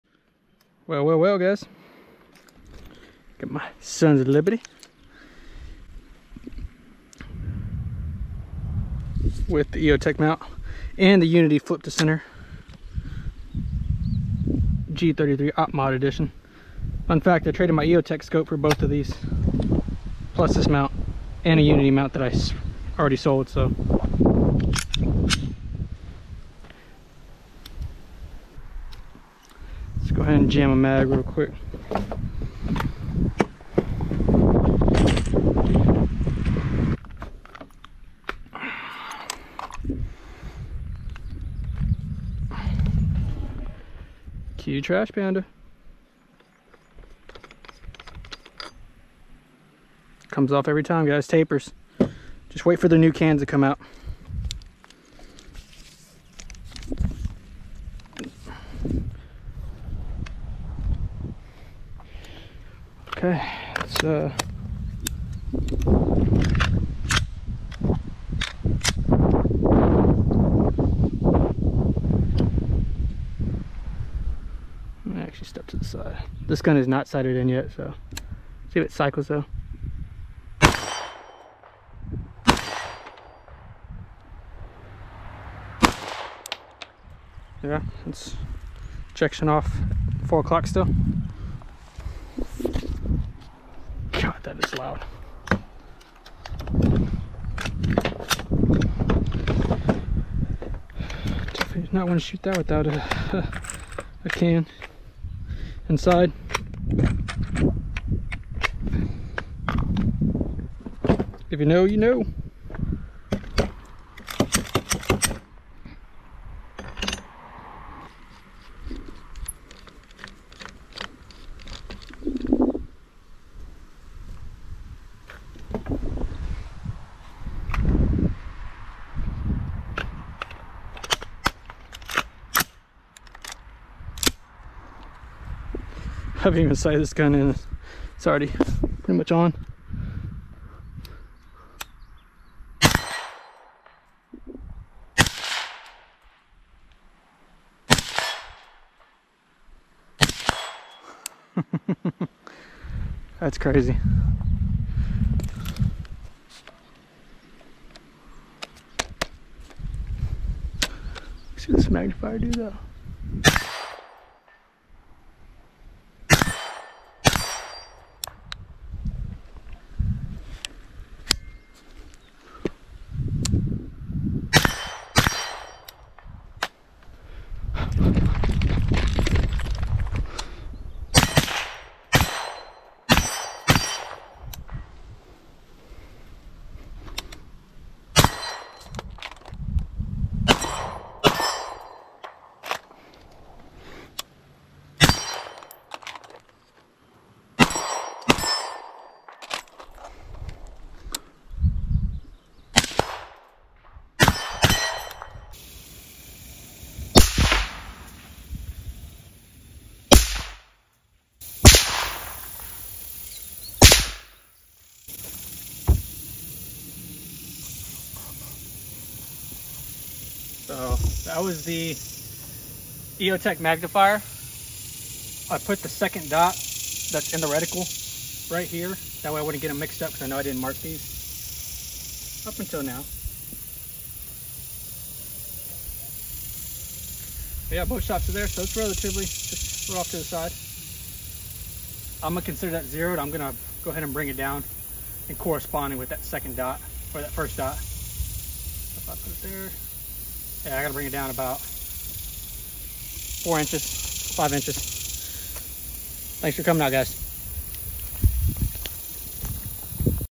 SCALAR WORKS LEAP 12 | UNITY FTC POV
THIS WAS SHOT A WHILE BACK UPON INSTALLING THE MOUNT,UPDATE WILL BE GOING OUT SOON,SO PLEASE DROP ANY QUESTIONS TO HELP OUT WHEN FILMING